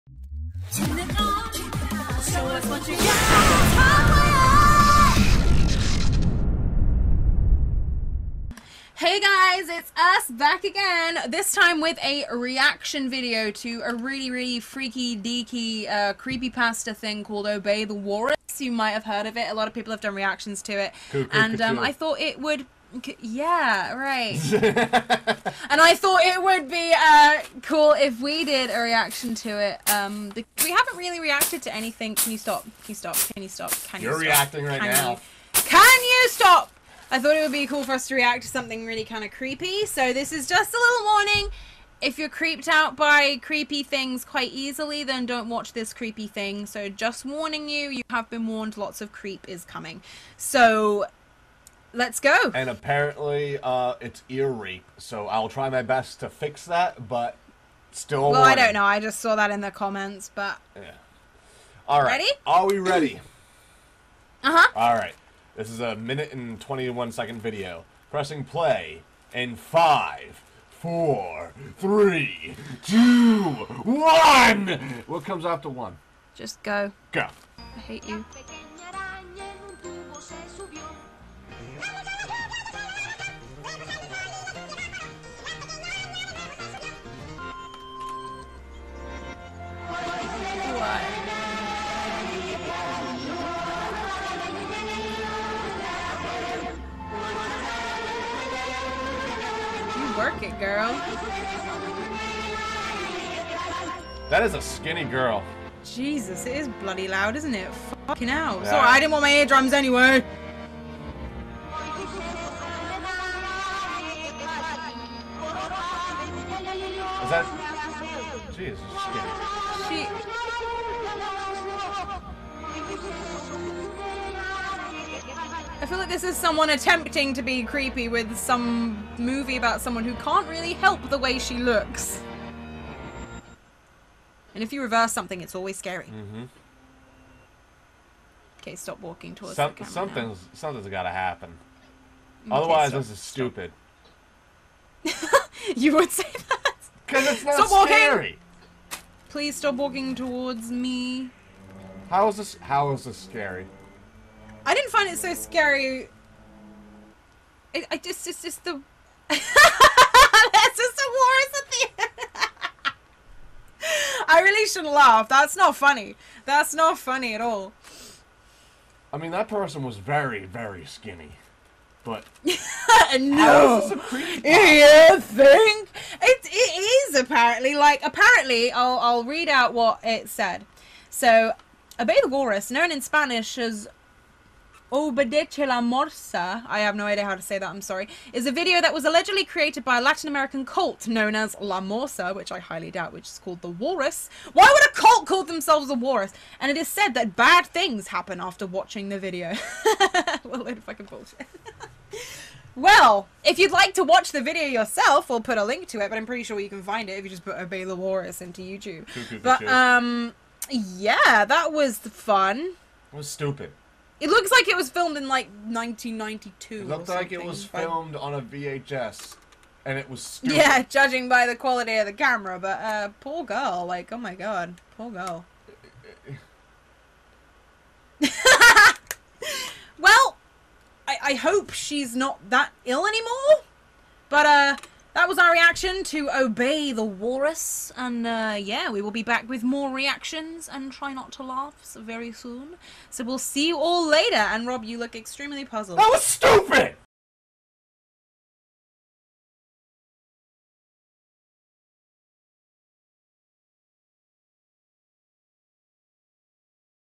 HOTWIRED REACTS! | Obey The Walrus | CREEPYPASTA
Elsie and Rob thought they'd check out this rather peculiar video, which they do. Will they be freaked out, or just slightly perturbed? Let's find out, shall we? Poor skinny person...

(Major derp in the thumbnail XD)